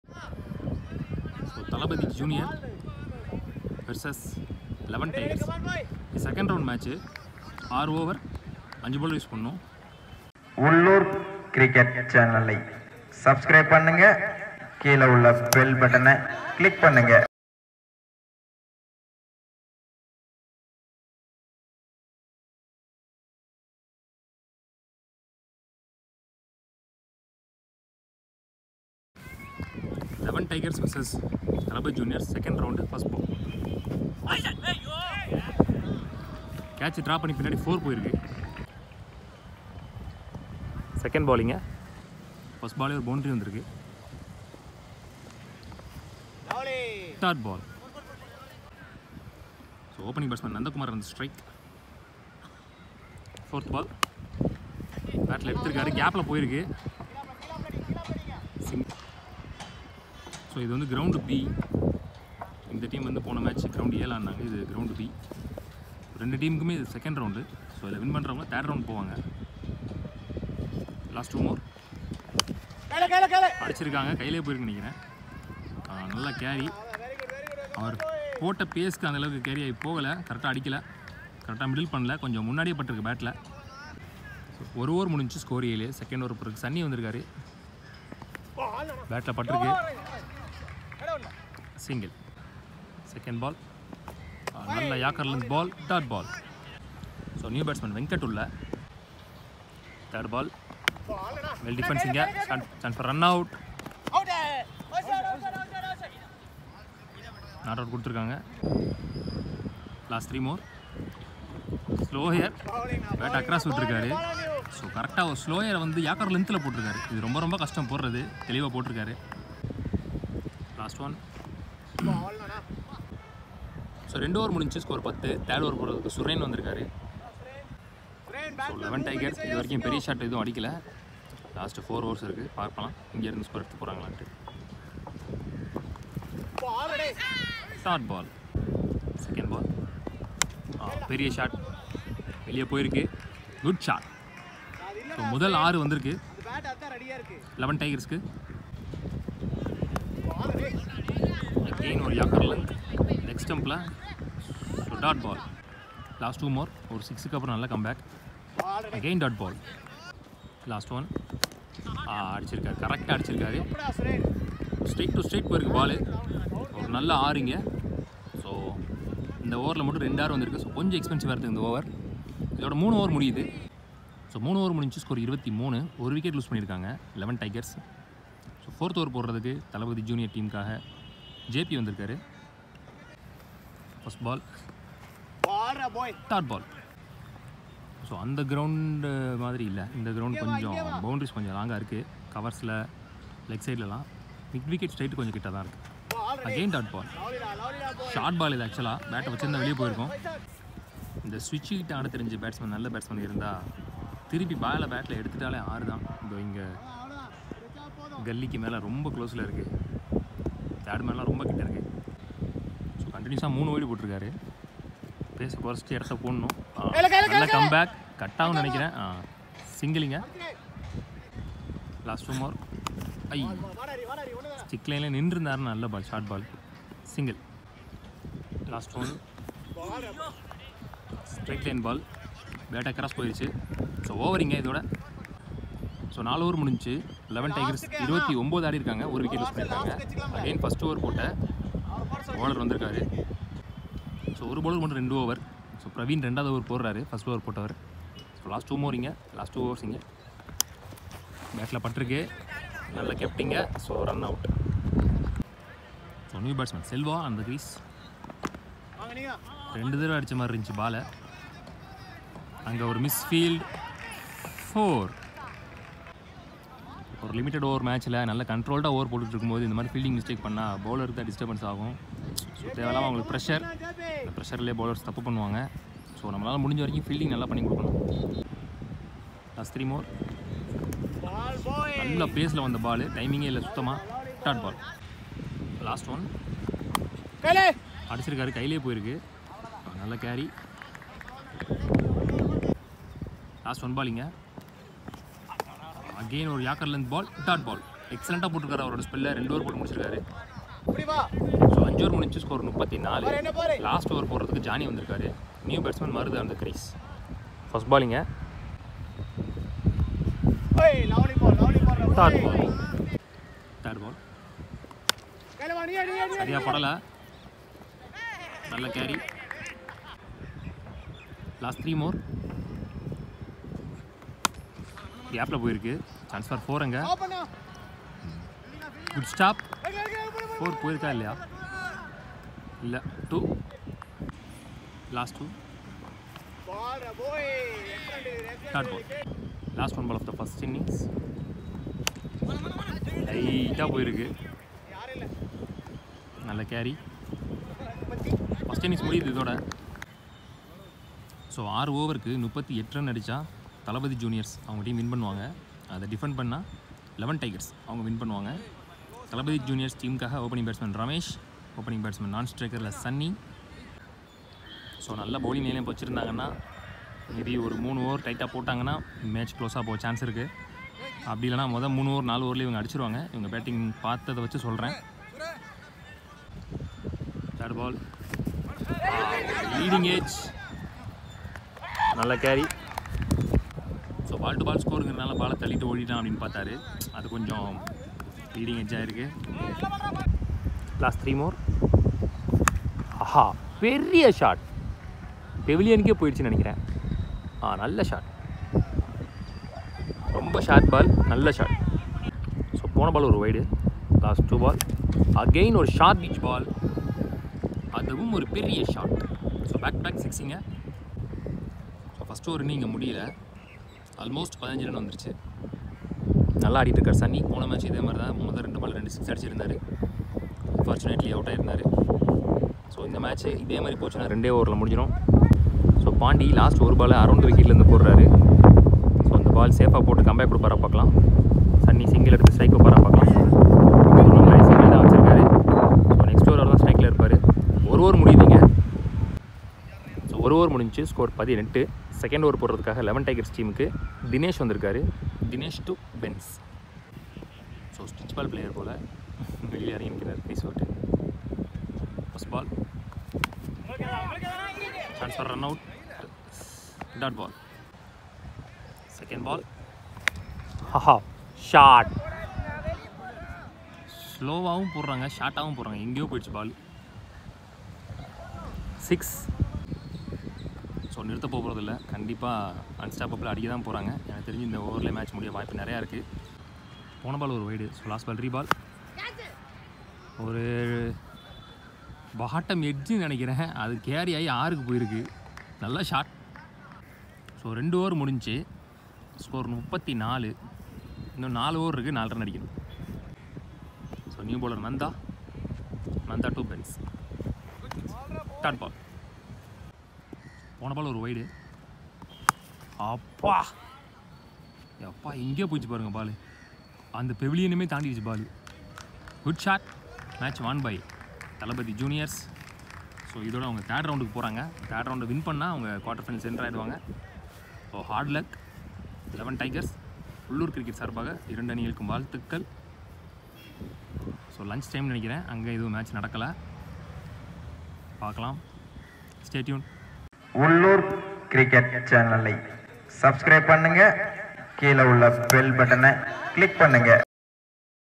Cricket | Coimbatore 1 Lakh Tournament | Thalapathy Juniors Vs 11 Tigers | ind vs nz test highlights
In this match between Thalapathy juniors vs 11 Tigers this is 6 over weight tennis ball match in psg hospital Coimbatore , 11 Tigers  batted first they started slowly and side by side they are loosing wickets so finally  sets a target 38 to the opponent team Thalapathy juniors

Thalapathy Juniors started good opening batsman Sanny and Benz  played a excellent innings and let see what happened.

Our Ultimate Goal is to Discover the Talented Youth Players From Tamilnadu.